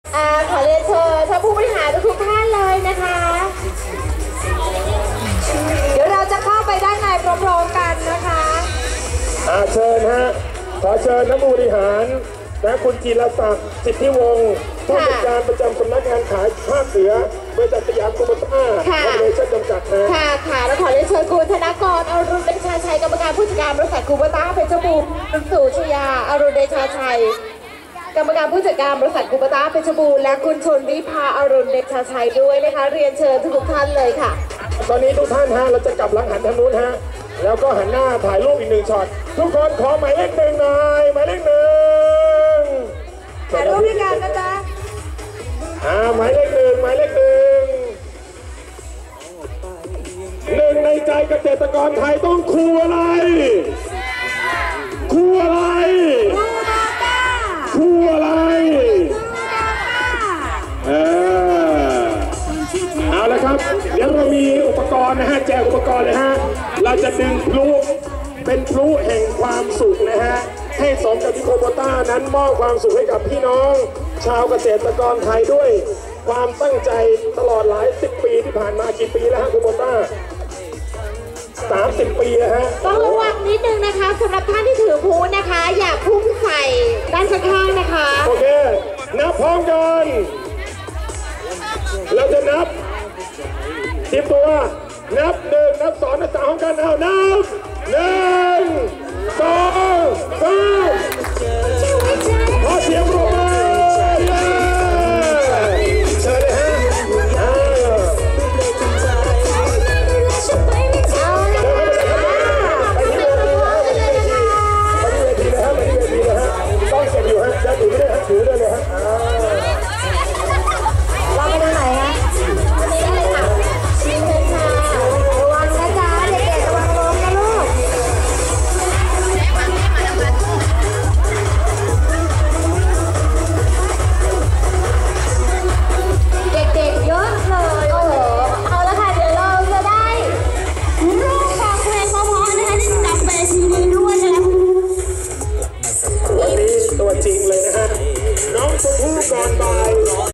108show ดาราชมพู่ ก่อนบ่ายกับพิธีกร เก่งไกรวิทย์
108show ดาราชมพู่ ก่อนบ่ายกับพิธีกร เก่งไกรวิทย์
งาน คูโบต้า ที่  1 ในใจเกษตรกรไทย ที่คูโบต้า สาขาหล่มสัก  จ.เพชรบูรณ์